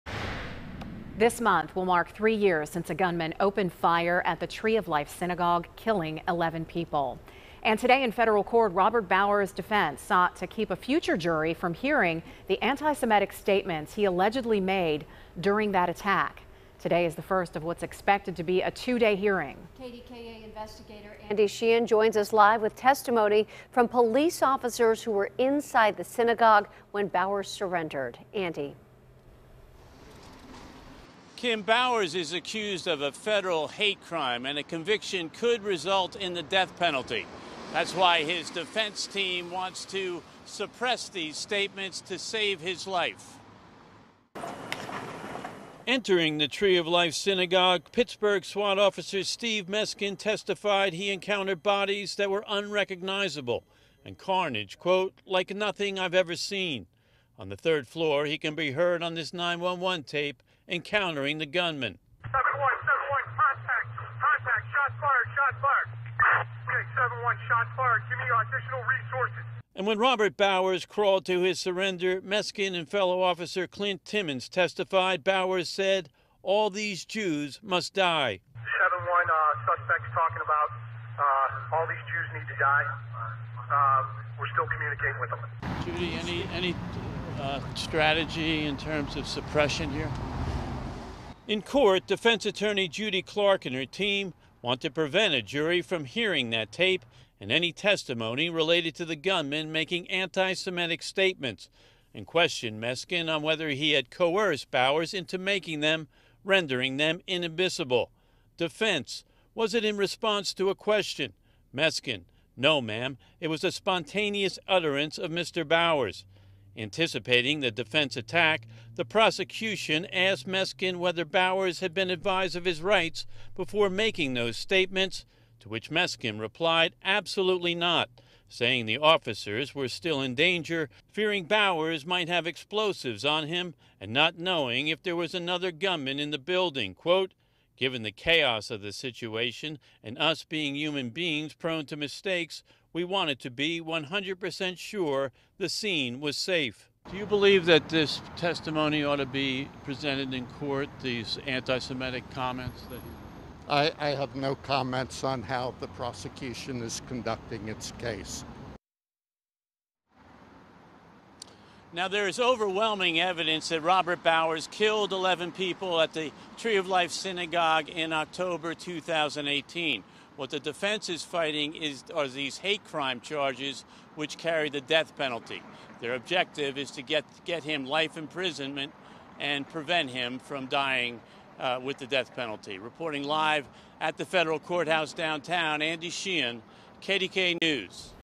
Officer: Accused Tree Of Life Synagogue Shooter Robert Bowers Talked Of Killing Jews
In federal court, Robert Bowers' defense sought to keep a future jury from hearing the anti-Semitic statements he allegedly made during the Tree of Life attack; KDKA's Andy Sheehan reports.